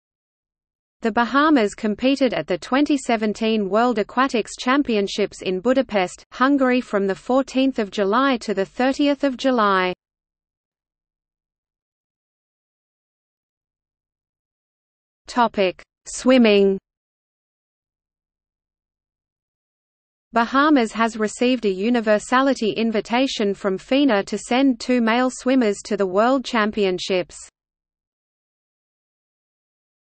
Bahamas at the 2017 World Aquatics Championships | Wikipedia audio article
00:00:11 Swimming

- Socrates

SUMMARY
The Bahamas competed at the 2017 World Aquatics Championships in Budapest, Hungary from 14 July to 30 July.